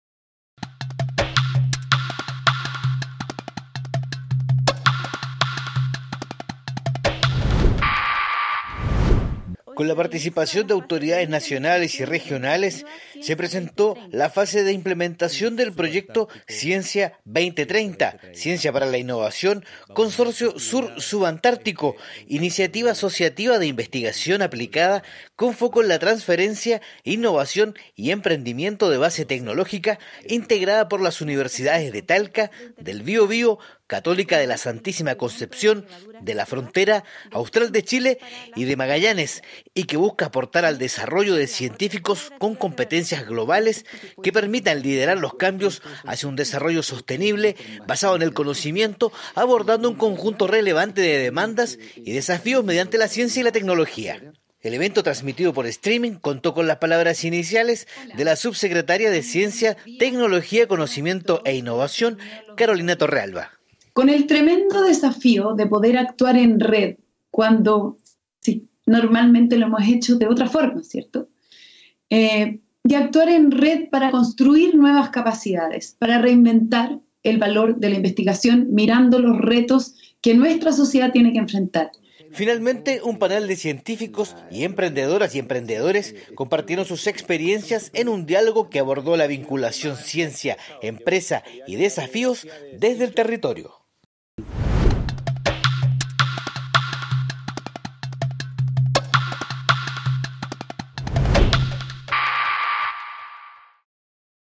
Lanzan fase de implementación del proyecto ciencia 2030, Consorcio Sur-Subantártico
Con la participación de autoridades nacionales y regionales se presentó la fase de implementación del proyecto Ciencia 2030, ciencia para la innovación Consorcio Sur - Subantártico, iniciativa asociativa de investigación aplicada con foco en la transferencia, innovación y emprendimiento de base tecnológica, integrada por las universidades de Talca, del Bio Bio, Católica de la Santísima Concepción, de La Frontera, Austral de Chile y de Magallanes, y que busca aportar al desarrollo de científicos con competencias globales que permitan liderar los cambios hacia un desarrollo sostenible, basado en el conocimiento, abordando un conjunto relevante de demandas y desafíos mediante la ciencia y la tecnología.

El evento transmitido por streaming contó con las palabras iniciales de la subsecretaria de Ciencia, Tecnología, conocimiento e Innovación Carolina Torrealba.